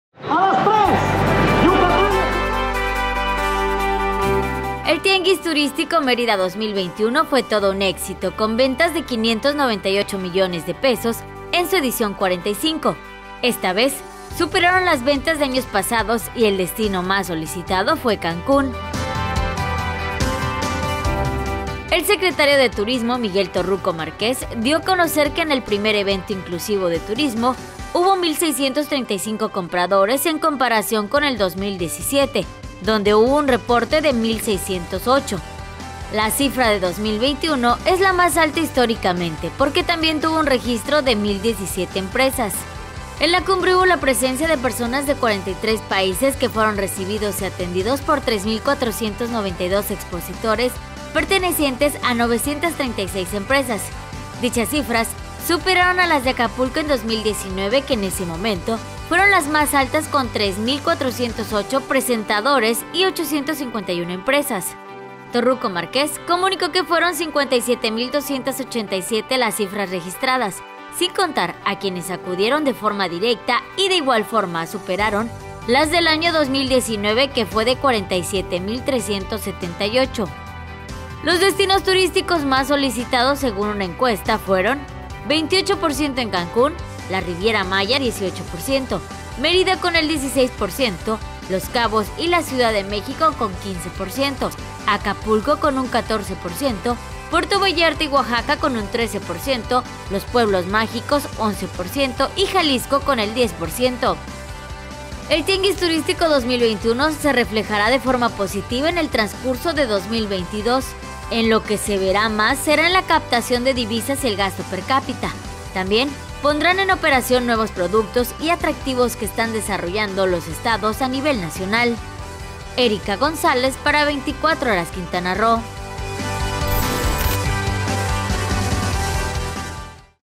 Quintana Roo rompe récords en el Tianguis Turístico 2021
TianguisTurísticoMérida2021 El evento del sector turístico más importante del país fue todo un éxito, las cifras hablan por sí solas.

📷 Erika González